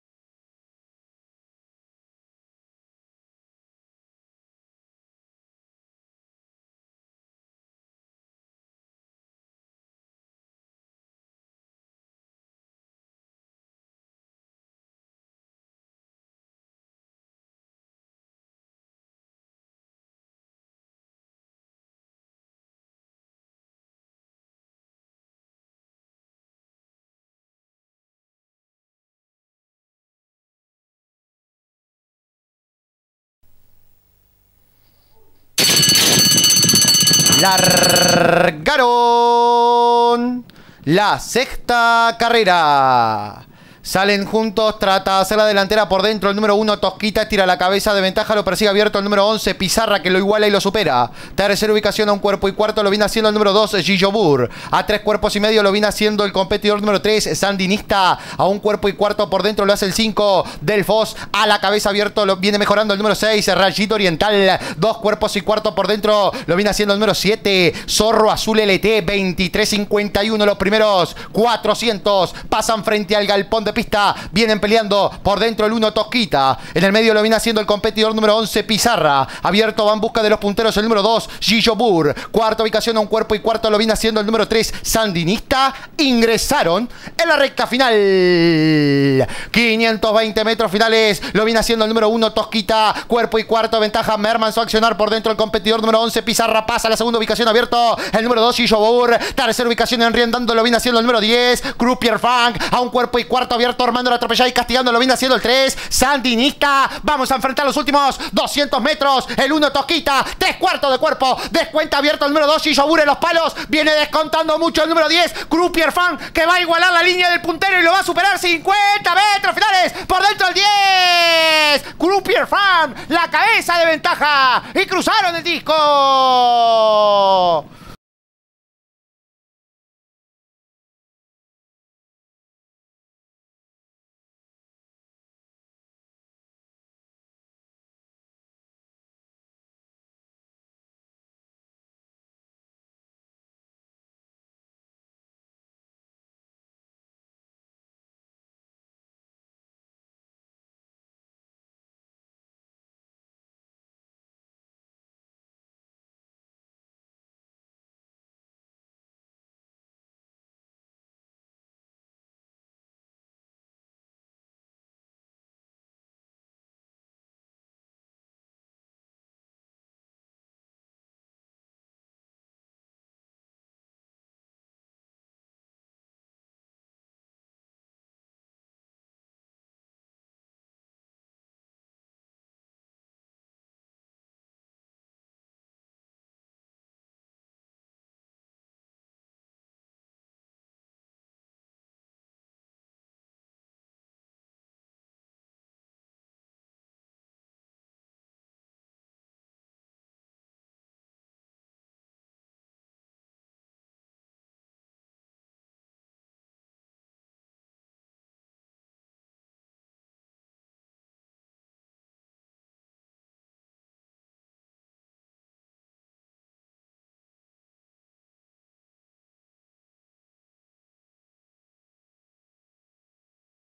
21/09/2018 – Hipodromo Maroñas – Carr 6 – TRASFOGUERO (2015)
Carrera 6 realizada el dia 21/09/2018 por el premio TRASFOGUERO (2015) en el Hipodromo Maroñas